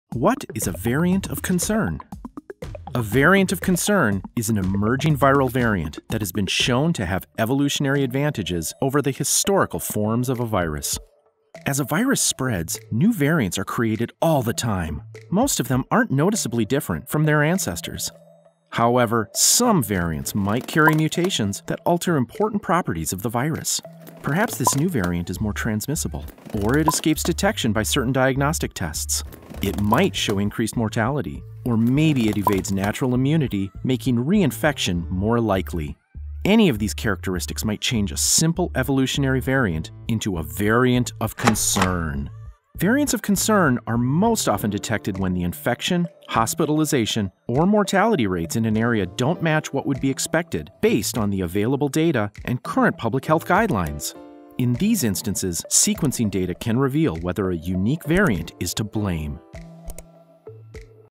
What Is a Variant of Concern?
Some viral variants are pretty harmless; sometimes new characteristics make a variant particularly deadly. When this happens, it could be labeled a "variant of concern".